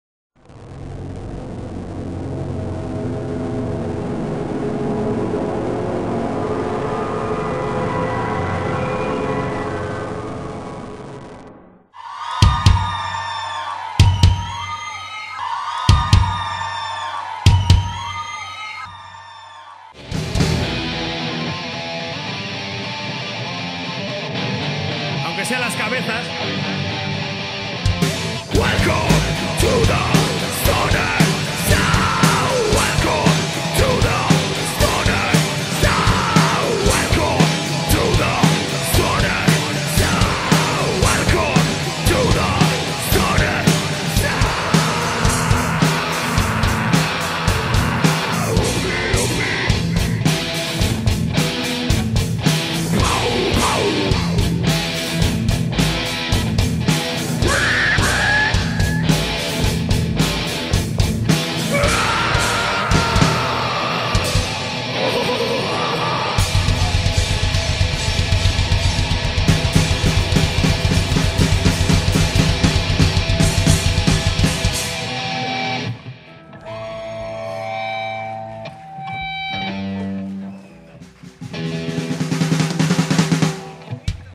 VÍDEO PROMOCIONAL CONCIERTO JIPLER BAND (TWISTED FATE)
AVANCE:
Pronto Video Completo del Concierto en el Hangar montado a multicamara.

By ModBer (Jose Luis Modroño Berdiñas).